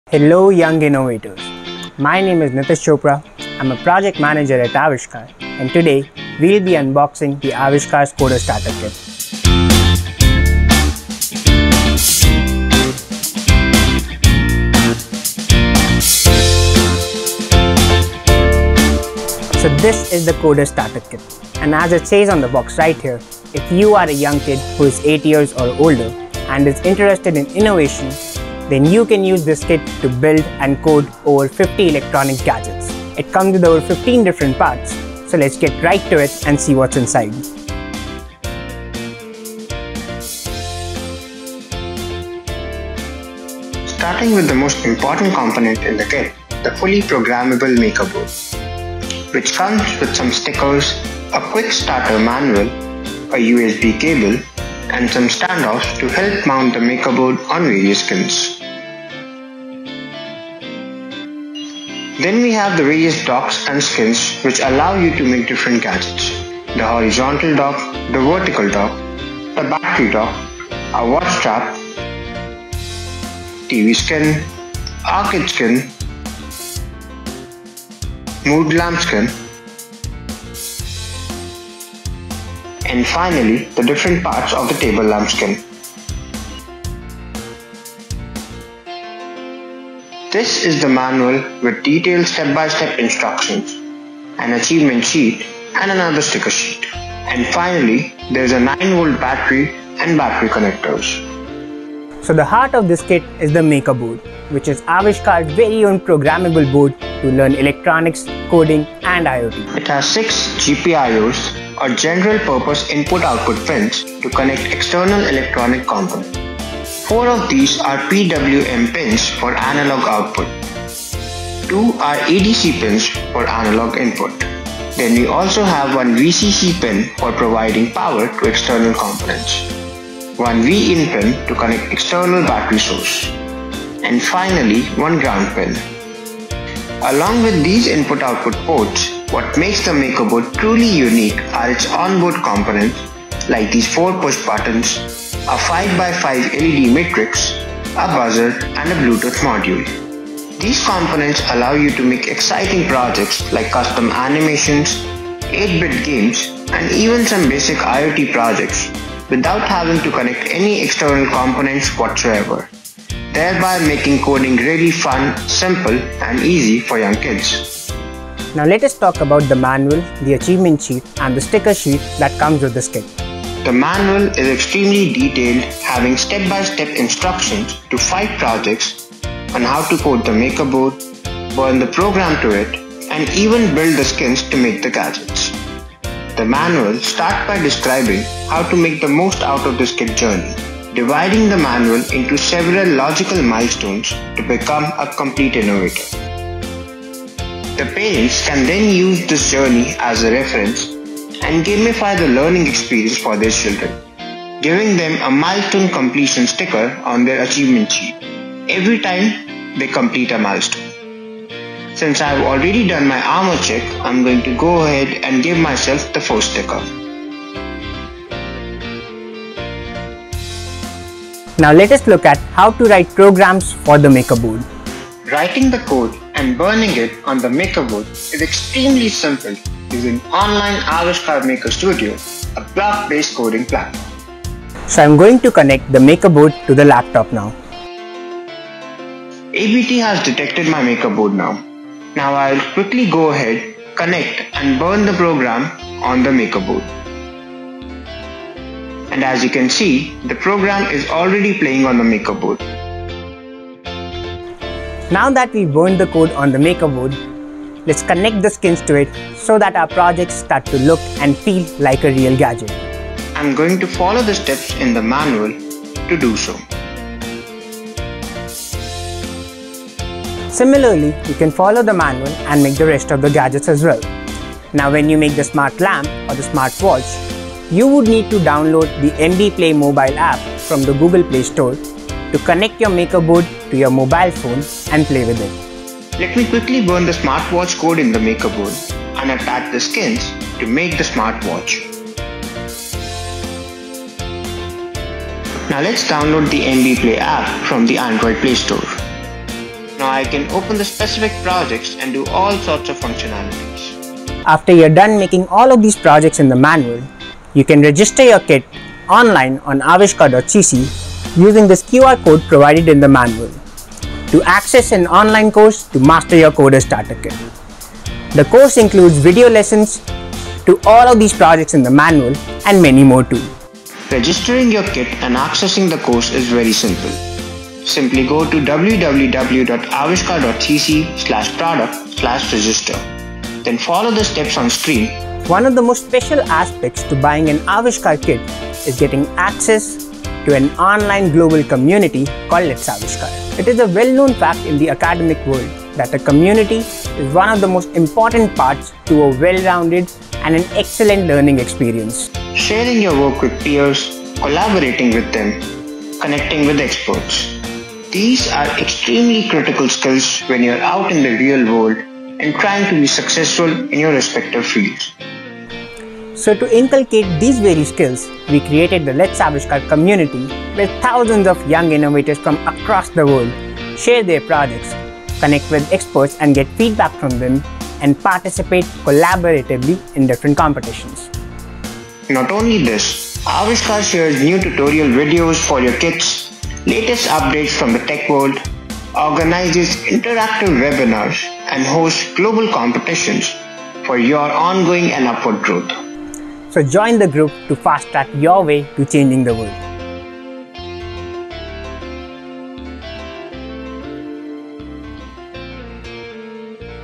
Coder's Starter Kit | Unboxing | Build And Code Over 50+ Electronic Gadgets
You have heard about it, you have seen it but we are giving you another sneak peek of our brand new Coder's Starter kit. You want to learn coding but something stopping you? Get your hands deep into coding with pre-made projects, an extensive manual, and other interesting, creative features.